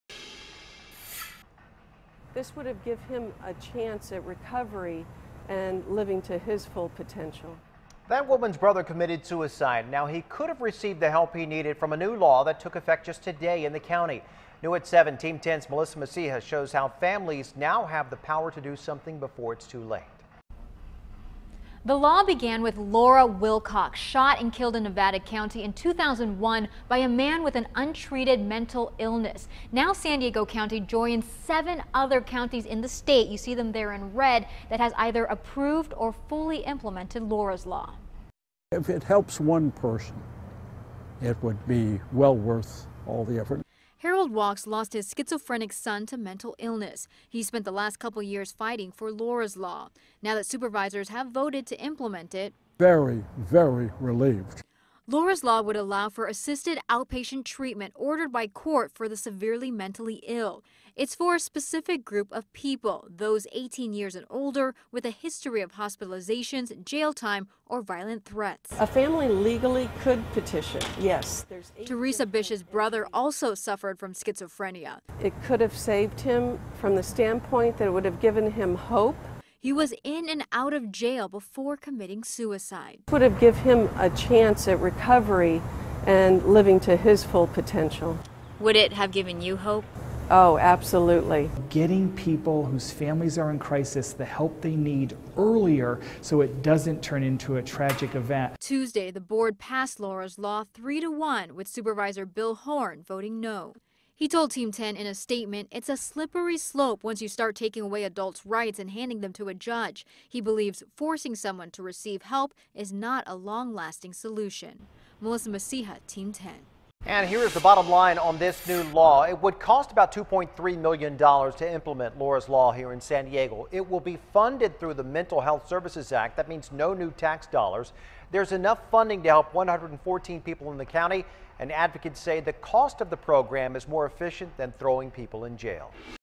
County votes to implement Laura's Law
Law allows court-ordered assisted outpatient treatment for mentally ill.